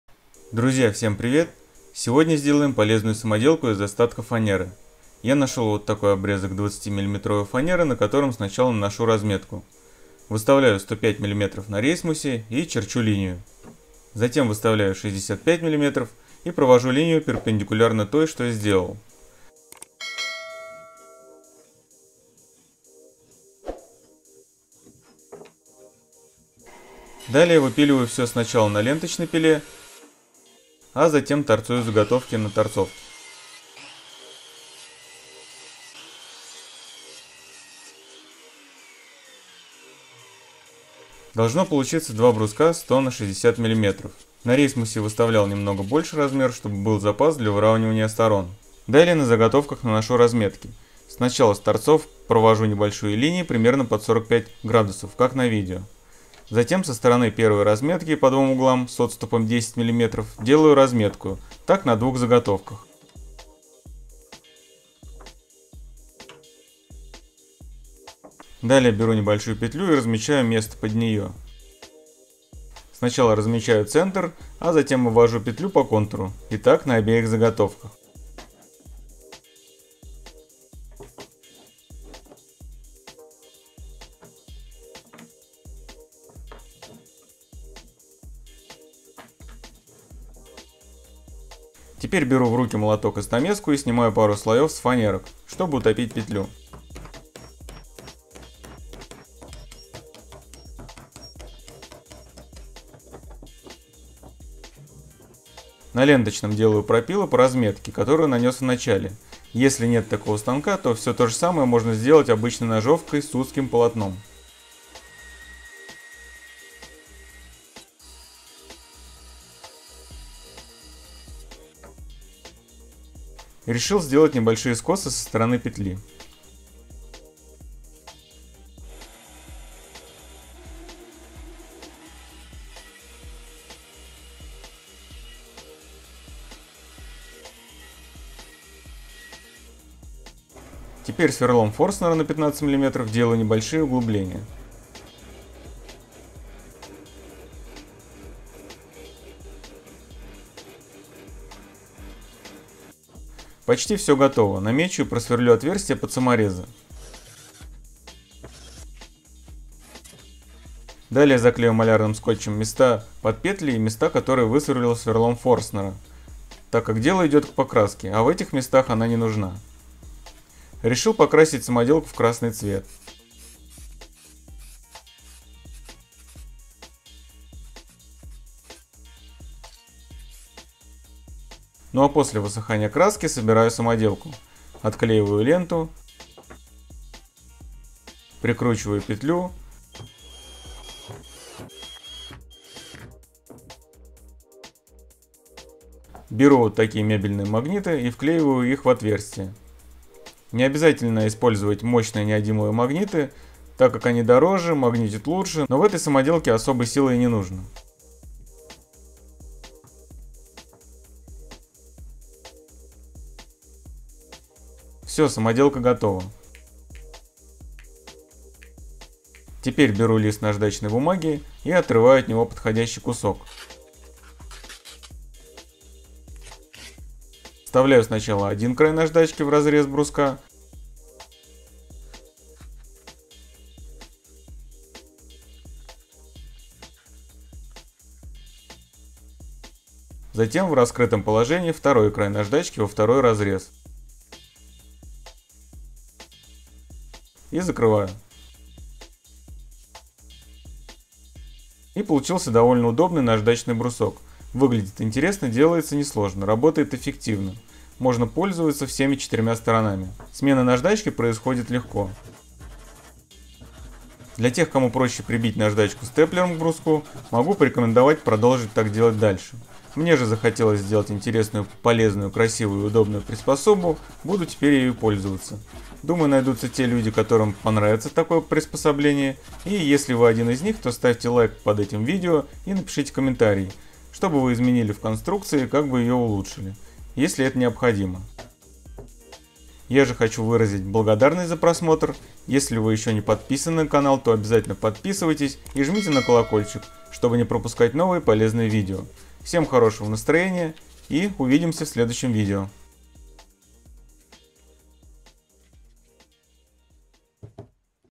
Идеальное использование остатков фанеры и дверной петли! Такого ты еще не видел!
Идеальное использование остатков фанеры и дверной петли! Такого ты еще не видел! Полезная самоделка для мастерской, пригодится каждому мастеру.